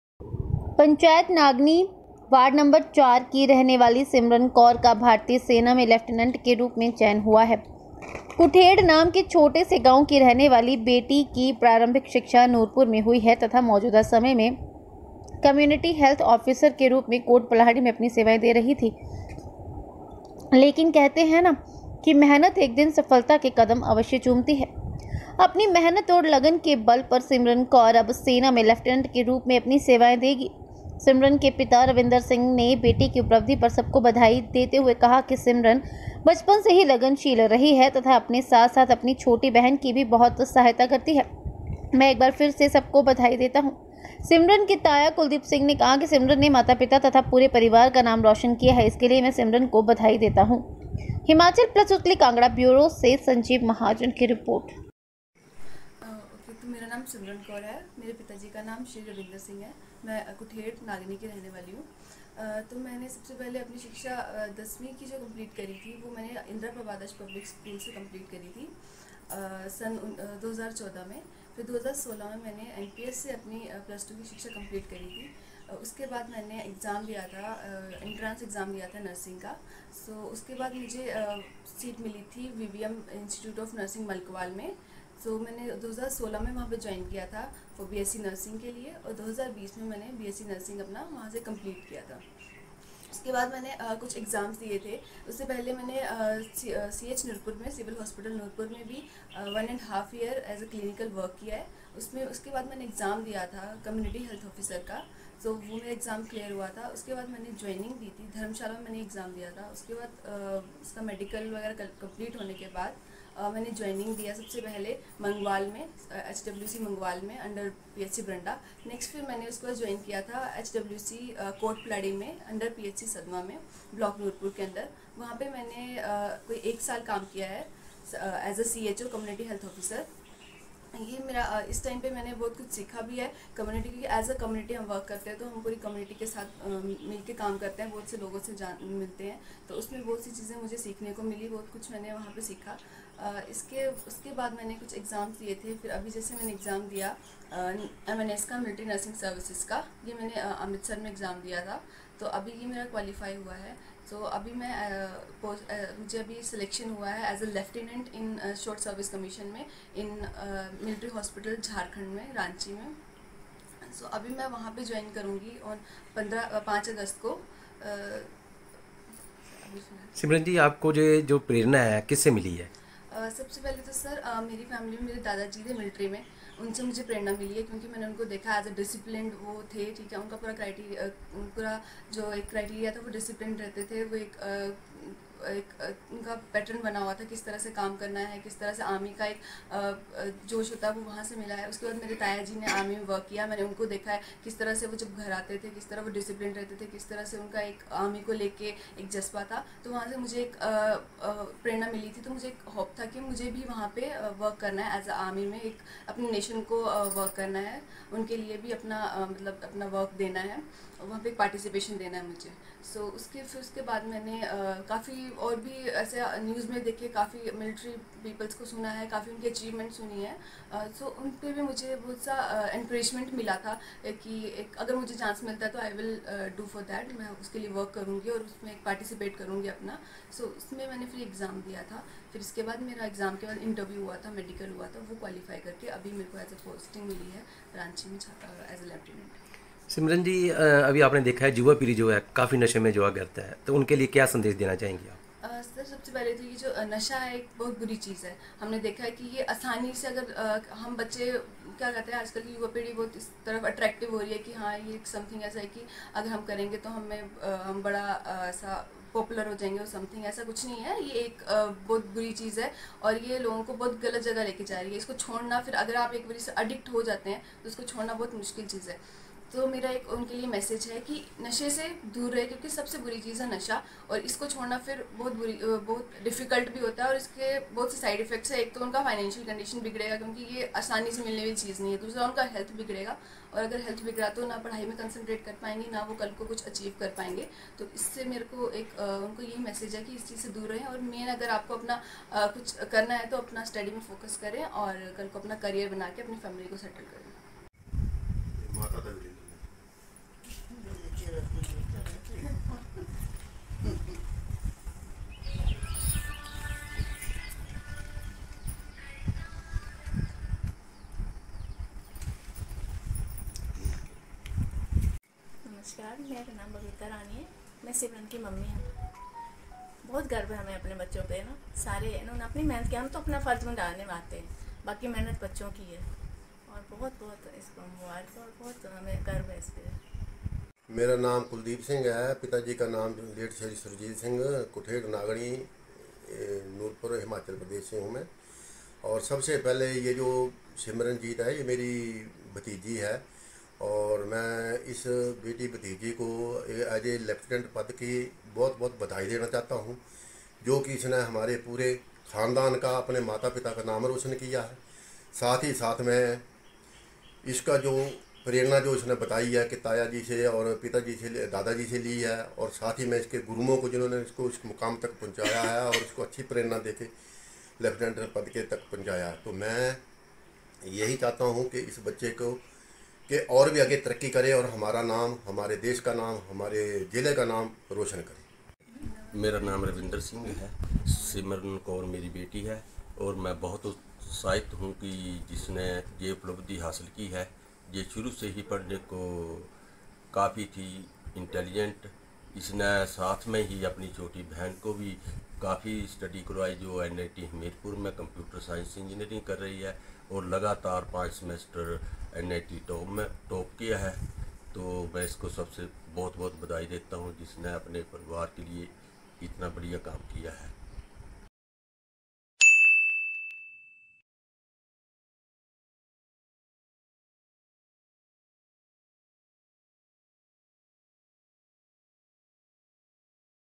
Nurpur की रहने वाली Simran Kaur का Indian Army में Lieutenant के रूप में चयन
Simran Kaur, resident of Noorpur, selected as Lieutenant in the Indian Army.

Keep Sharing

Keep Supporting